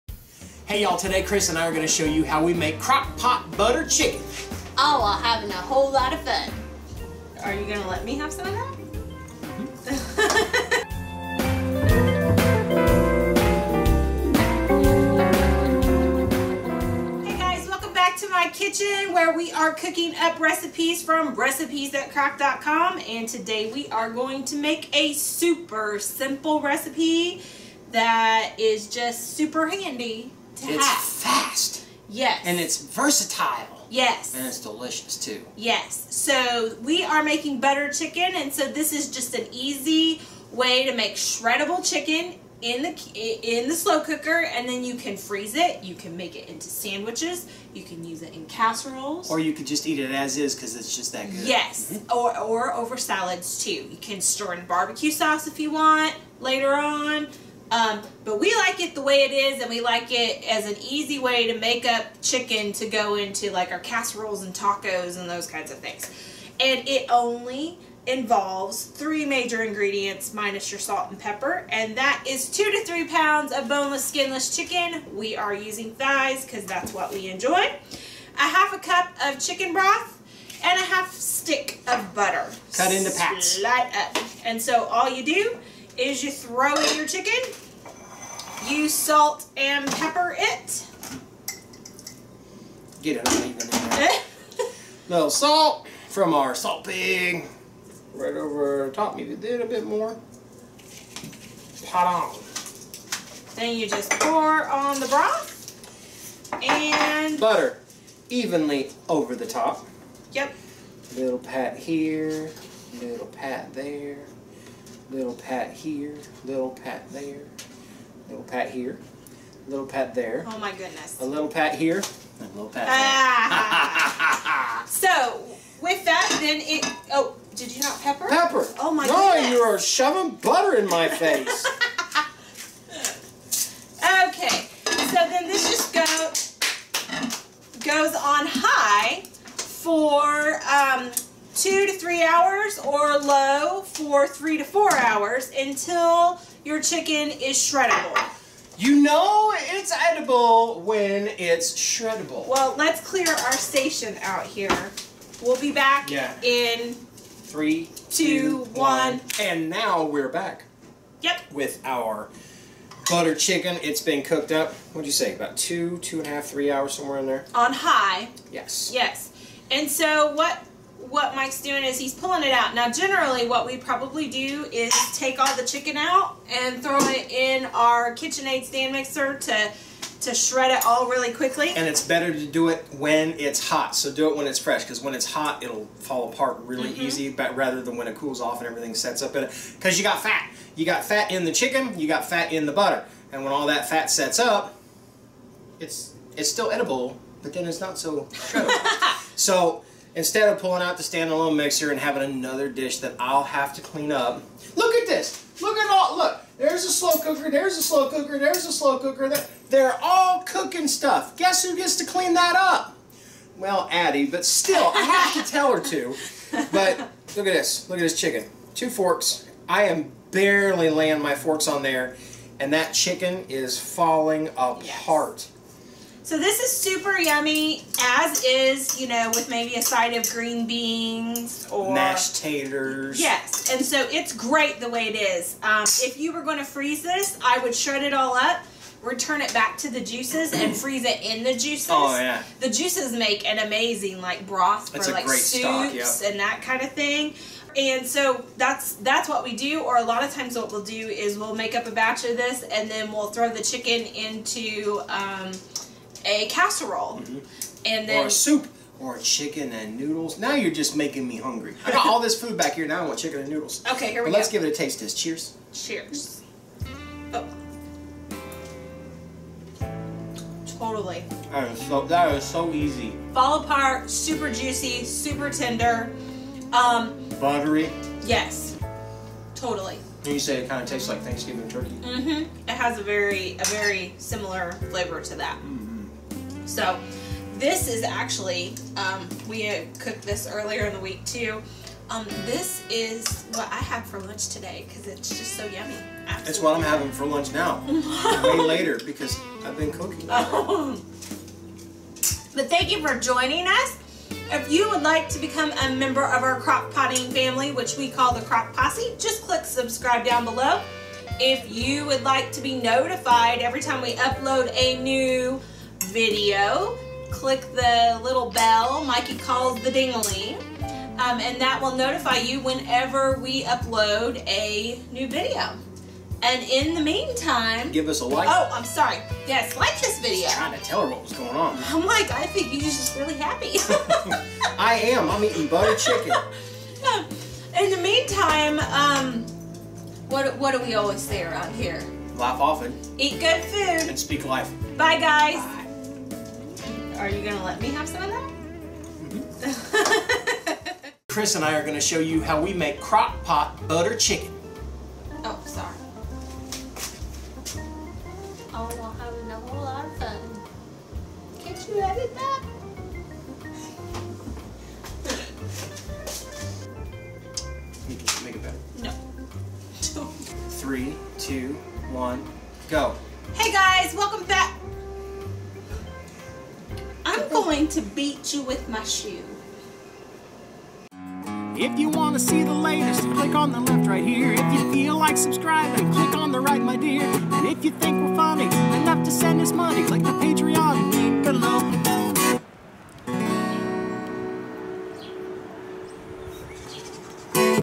Crock Pot Butter Chicken
You can't get much easier or more versatile that this folks!  The Crock Pot Butter Chicken recipe can be eaten as is, shredded, put on salads, casseroles, tacos...you name it! It's SO GOODe!!!

(BIG thanks to all our supporters!)

T-shirts & Mugs & Aprons OH MY!!!

We LOVE hearing from y'all!!!

Fan Mail:
Recipes That Crock
Monrovia, IN 46157